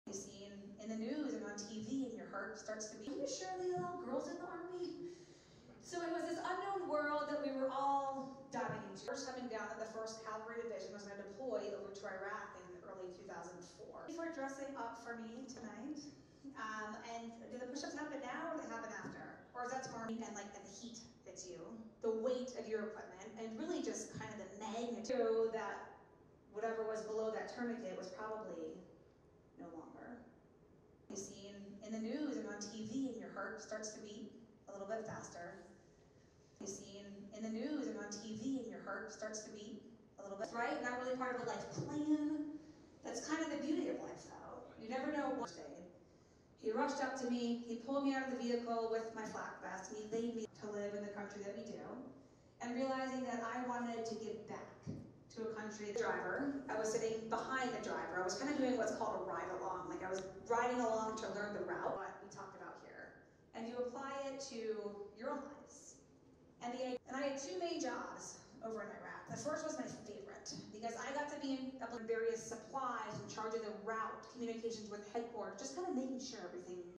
B-Roll of Paralympian and Iraq War Amputee Melissa Stockwell.
Paralympian and Iraq War Amputee Melissa Stockwell spoke at Central Washington in Ellensburg.